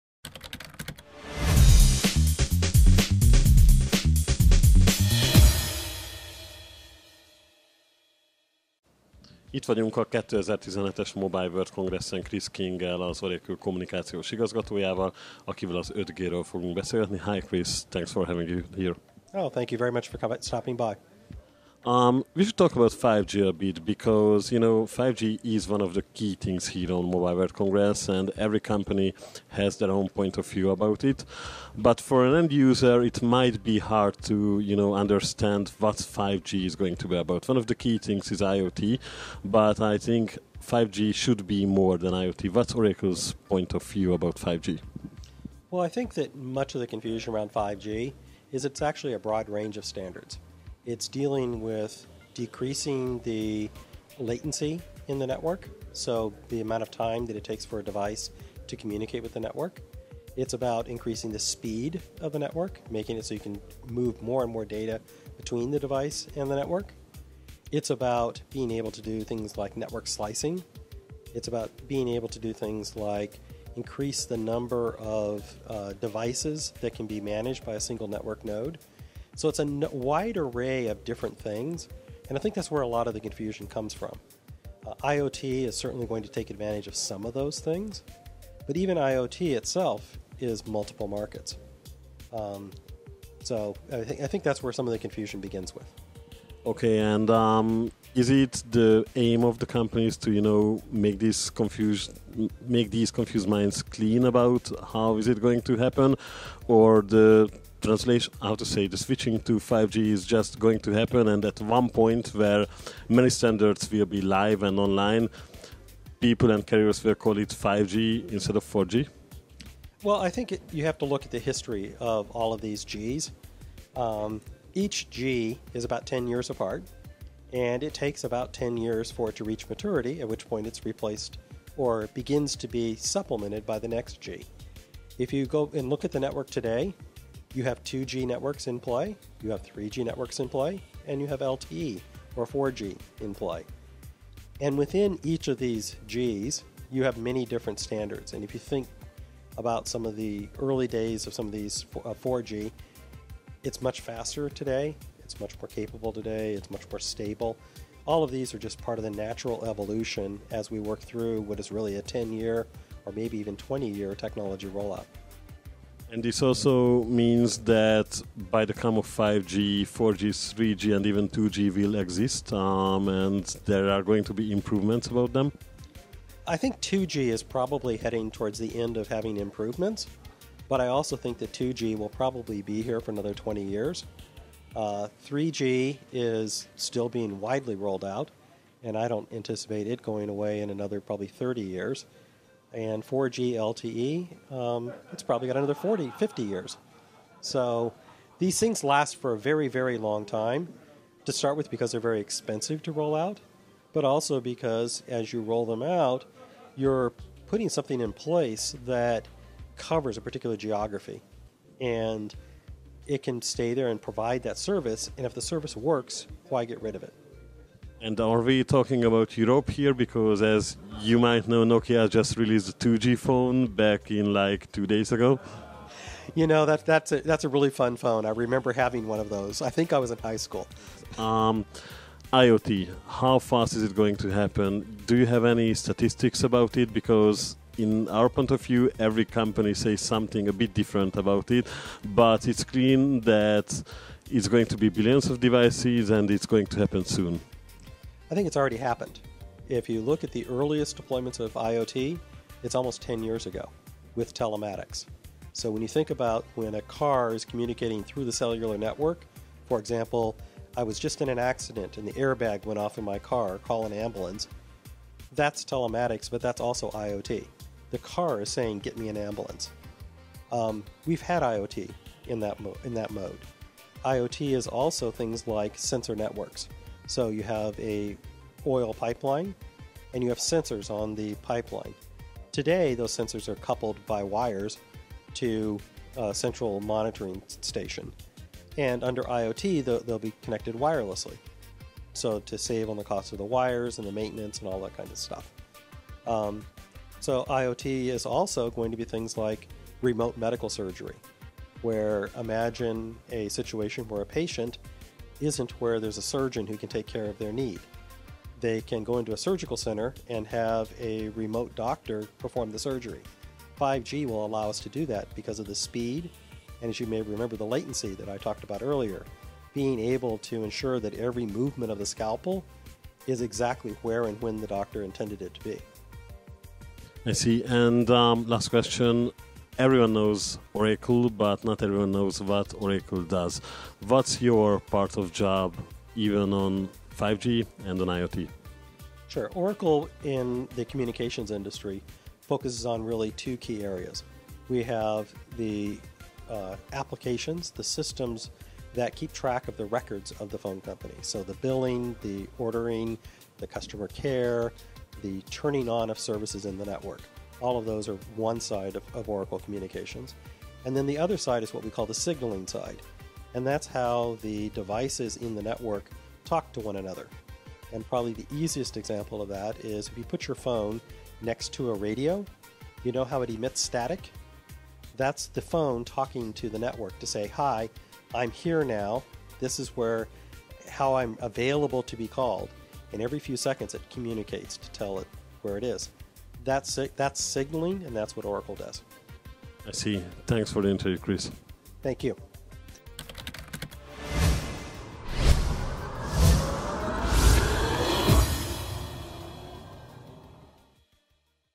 MWC 2017: Az 5G-ről és az IoT-ről beszélgettünk az Oracle-lel
A mobil világkiállításon ellátogattunk az Oracle-höz, ahol a kommunikációs igazgatóval beszélgettünk az 5G-ről, egy kicsit más megvilágításban, mint korábban. Fejtágítás alapszinten.